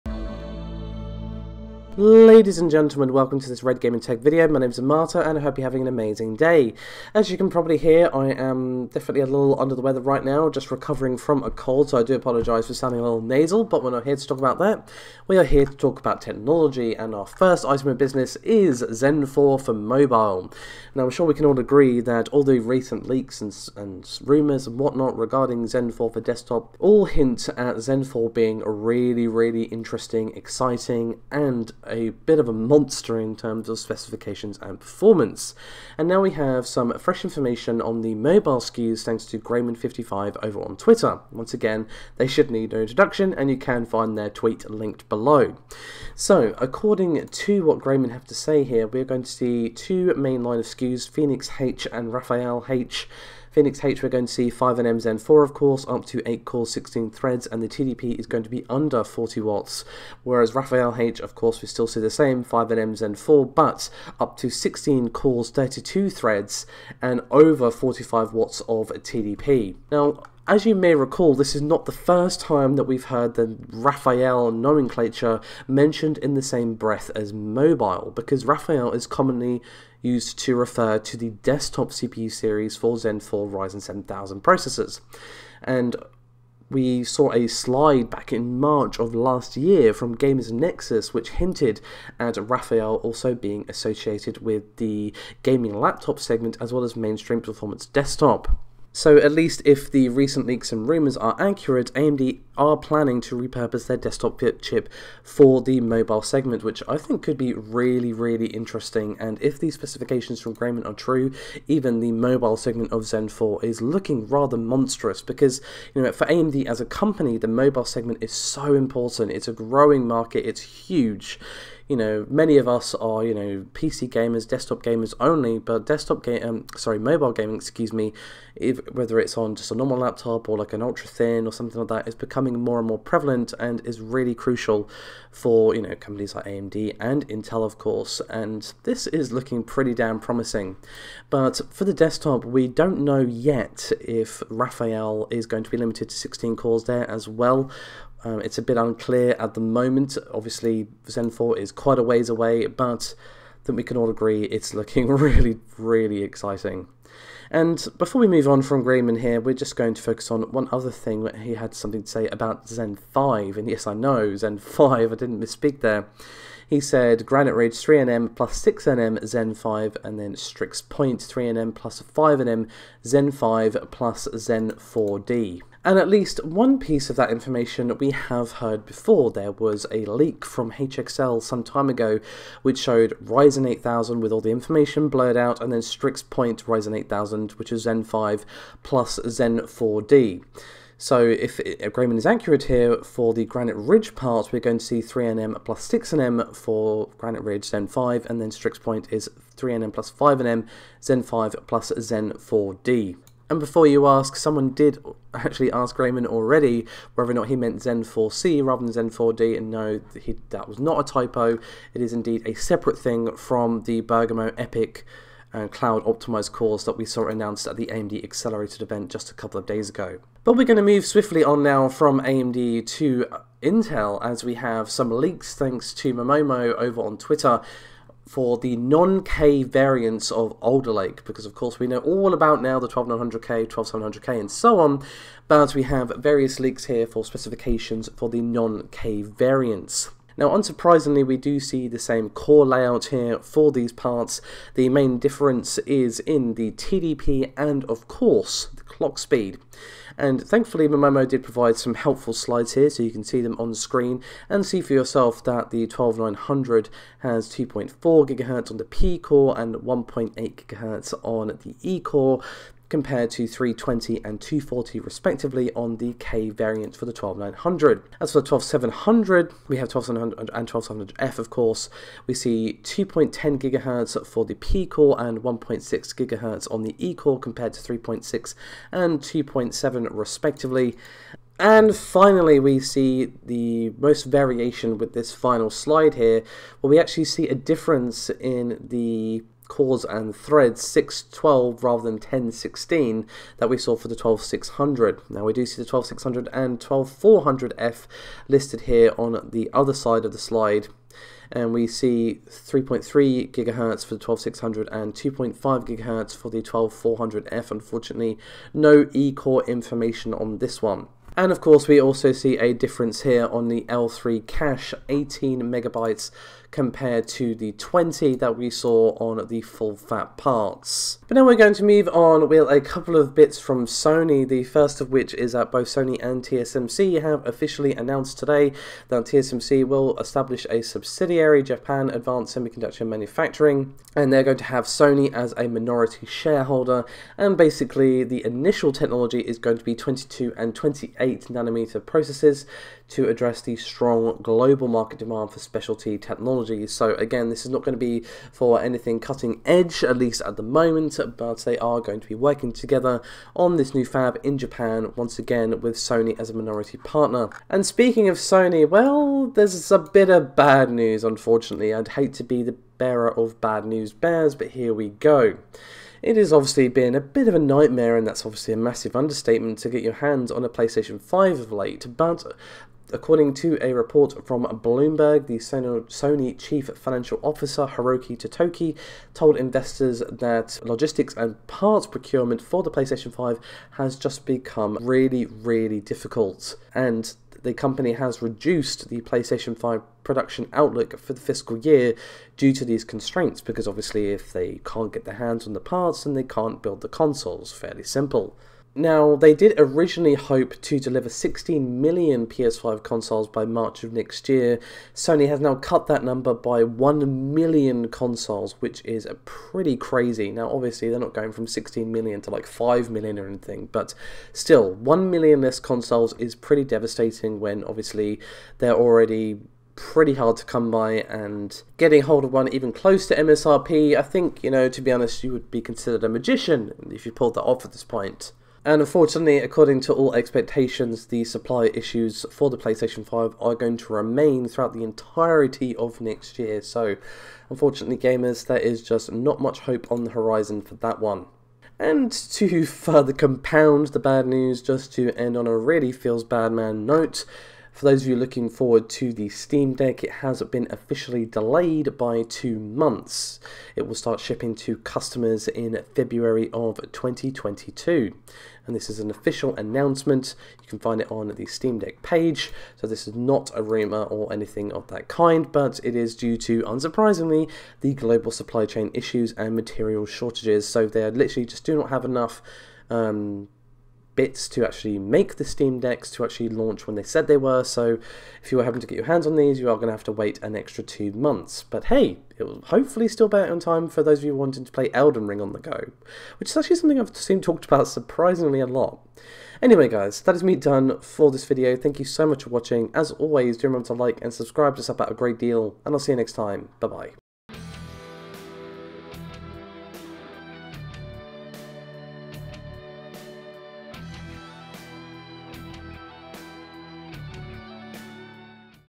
Zen 4 Mobile to DECIMATE Your Desktop CPU | Sony CUTS PS5 Production
Some specs have leaked for Zen 4 APUs for mobile, and if these Zen 4 mobile specifications are accurate, it is set to DECIMATE your current gen desktop CPU. We examine the leaked specs for Zen 4 Raphael H and Zen 4 Phoenix H, two SKU lines we can apparently expect AMD to release for laptops and ultra thin gaming as part of Ryzen 7000 processors. Will Intel's upcoming Raptor Lake APU / CPU series for mobile be able to compete with the raw hardware specs and gaming performance of Zen 4 mobile processors?

Sony have also CUT their estimates for PS5 production, as they struggle with supply chain shortages for hardware parts of the Playstation 5. Given that PS5 stock is very hard to get hold of amongst the shortages, less production (and less Playstation 5 next gen consoles  hitting shelves) is the last thing Sony gamers want.

We also discuss Intel Alder Lake non-K CPU specs which have leaked, Zen 5 Graphite Ridge and Valve's upcoming Steam Deck release date delay.

0:00 Start
0:20 Zen 4
2:43 Zen 5
3:45 Alder Lake non-K
5:24 Sony TSMC
5:57 PS5 production
7:27 Steam Deck